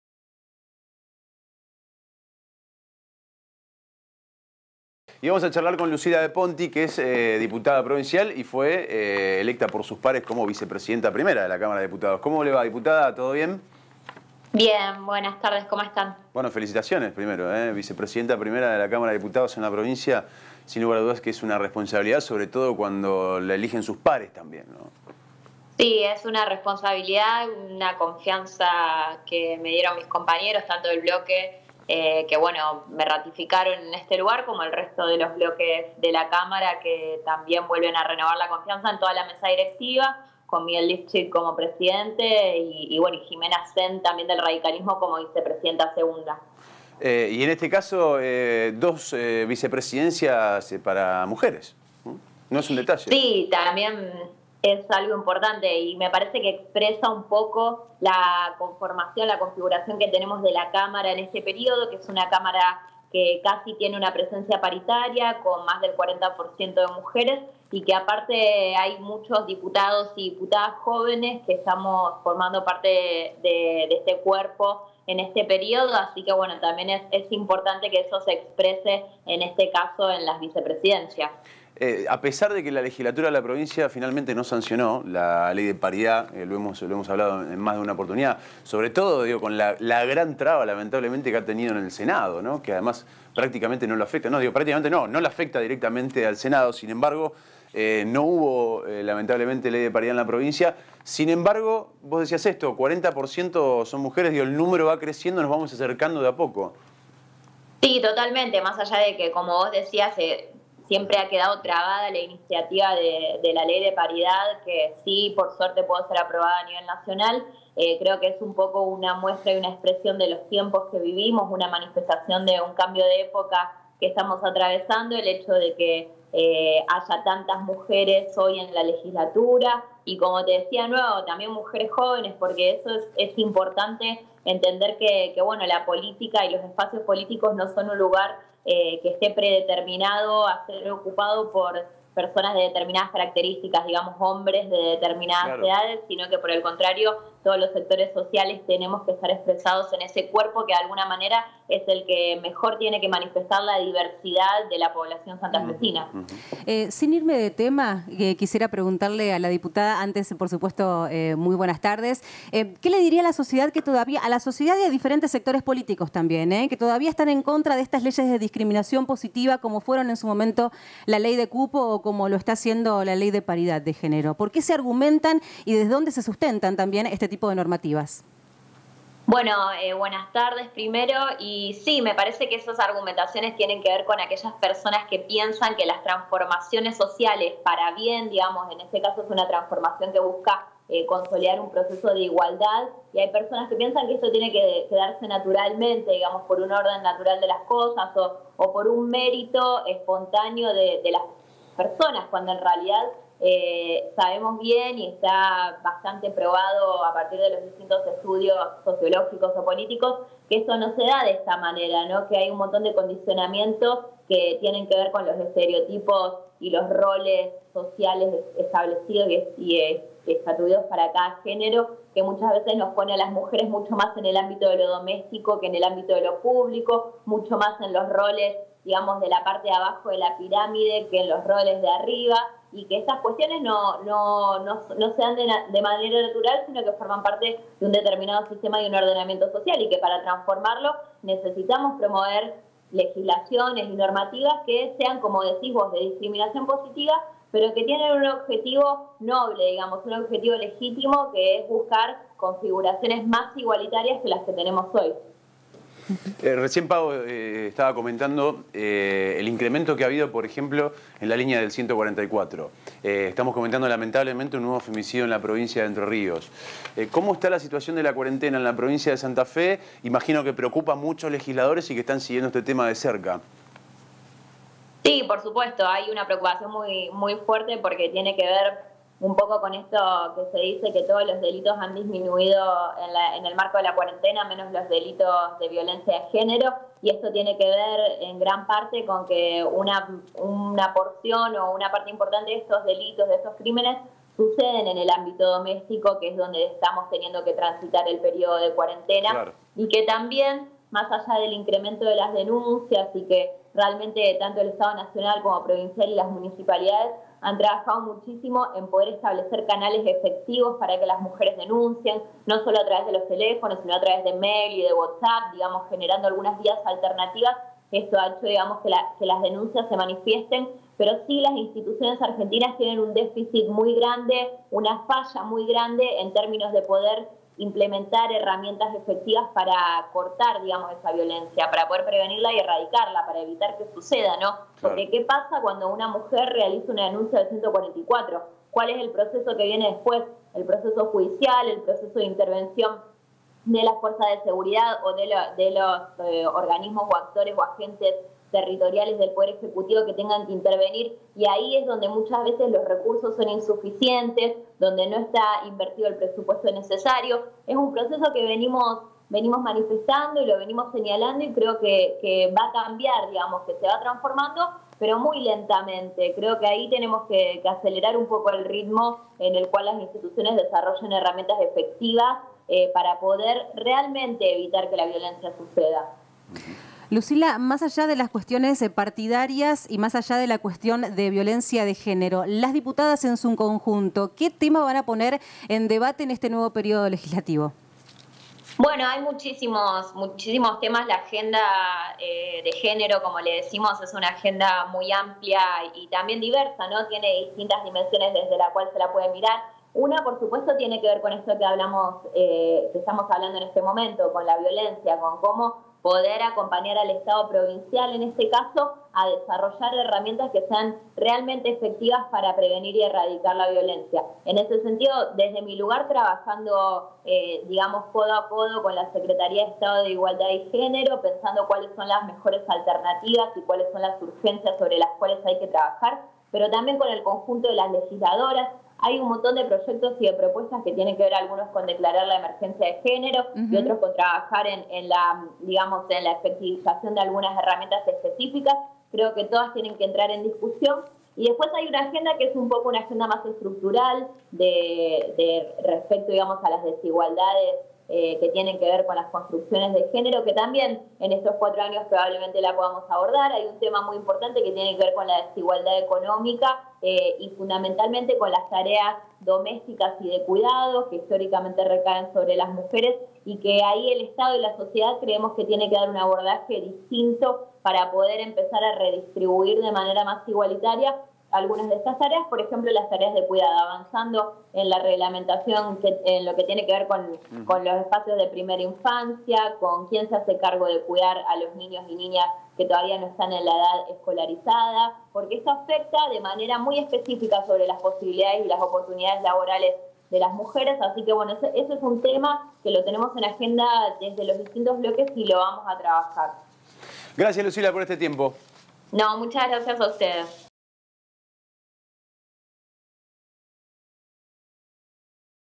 Lucila De Ponti - Diputada Provincial (PJ) - ATP 27 04 20
Ley de Paridad y actividad legislativa en medio de la pandemia